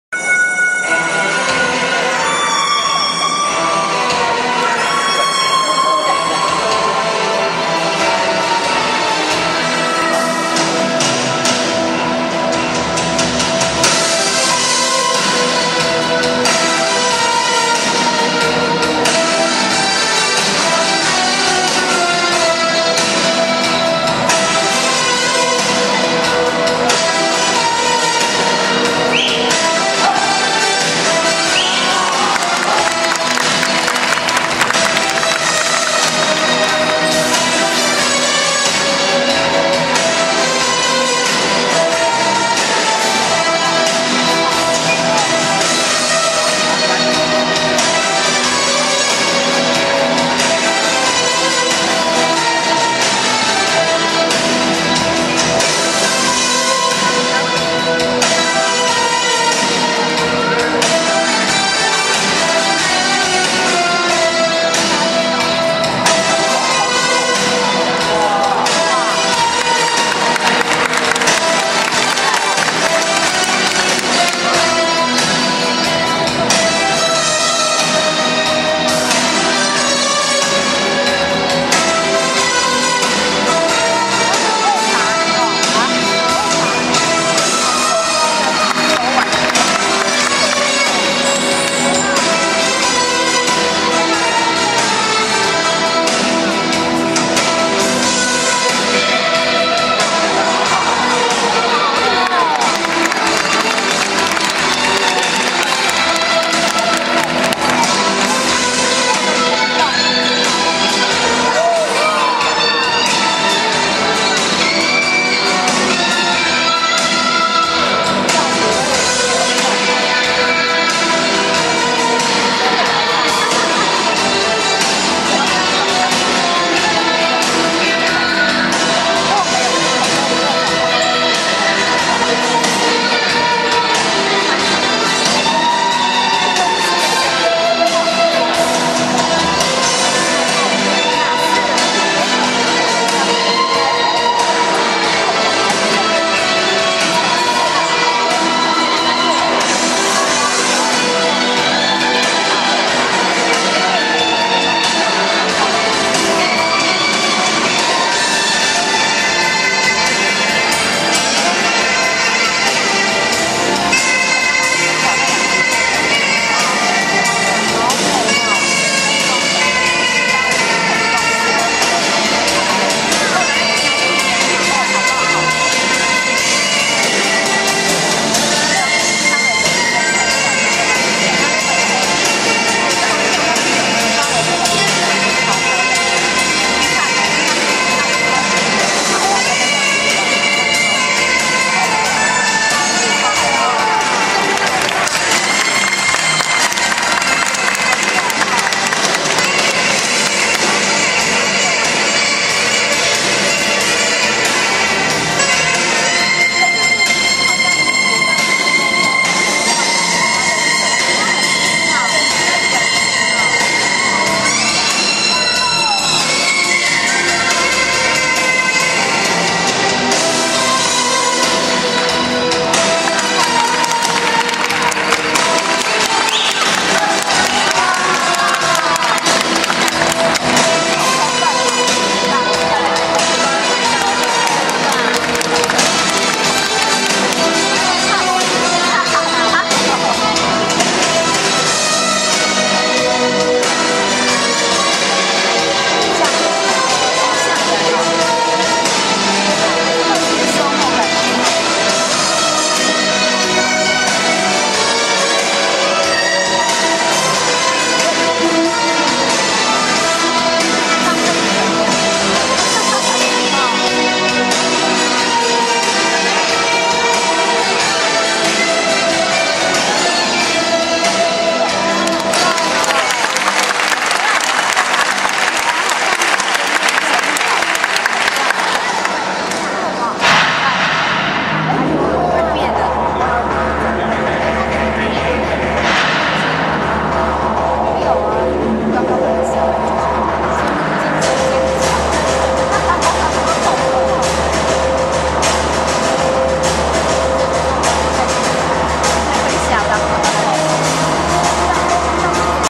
CHINESE POLE ACT BY LIMPOPO MALINDI AFRICAN ACROBATS
DOUBLE CHINESE POLE ACT PERFORM BY THE MALINDI FLYING ACROBATS. THE ACT CAN ALSO BE DONE IN AFRICAN COSTUMES AND MUSIC WITH AFRICAN DANCING.  GROUP TRAINED BY XAVIER JOAO THIMO